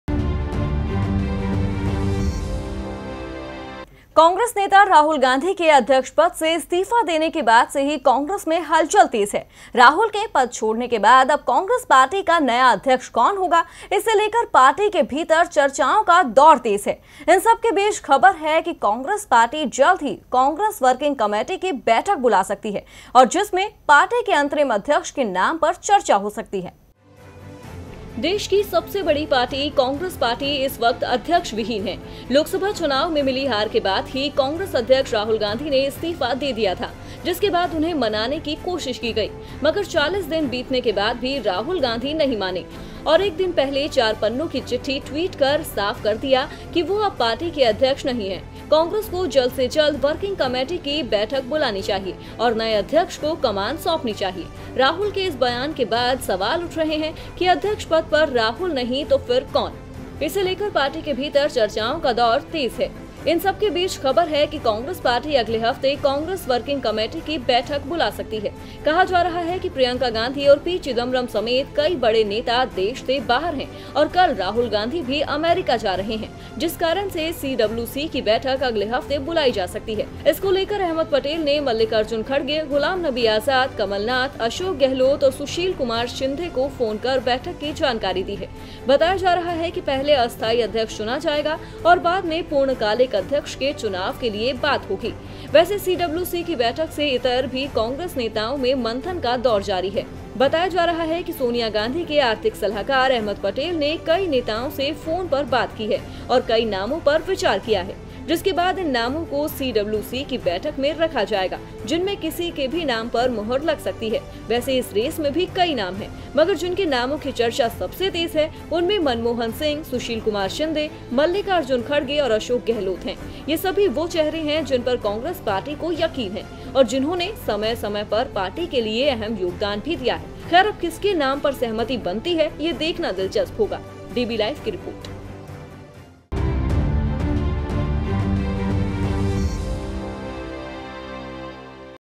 जल्द मिलेगा कांग्रेस को नया अध्यक्ष! |अगले हफ्ते हो सकती है CWC की बैठक
कांग्रेस नेता राहुल गांधी के अध्यक्ष पद से इस्तीफा देने के बाद से ही कांग्रेस में हलचल तेज है...राहुल के  पद छोड़ने के बाद अब कांग्रेस पार्टी का नया अध्यक्ष कौन होगा? इसे लेकर पार्टी के भीतर चर्चाओं का दौर तेज है...इन सबके बीच खबर है कि कांग्रेस पार्टी जल्द ही कांग्रेस वर्किंग कमेटी की बैठक बुला सकती है...और जिसमें पार्टी के अंतरिम अध्यक्ष के नाम पर चर्चा हो सकती है।